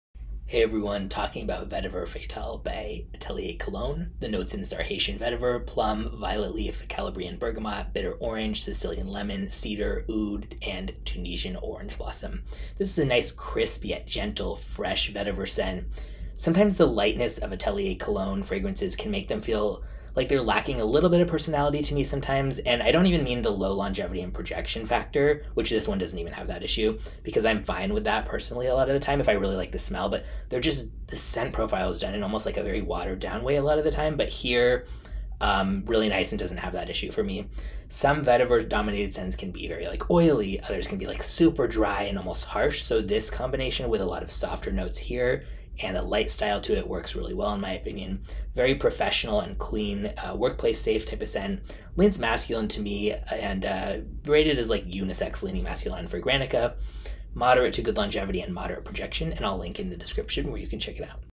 Atelier Cologne Vetiver Fatal Review | Nice Crisp Vetiver - Professional & Clean | Unisex/Masculine
Review of Vetiver Fatal by Atelier Cologne.

The notes in this fragrance are:

Haitian vetiver, plum, violet leaf, calabrian bergamot, bitter orange, sicilian lemon, cedar, agarwood (oud), tunisian orange blossom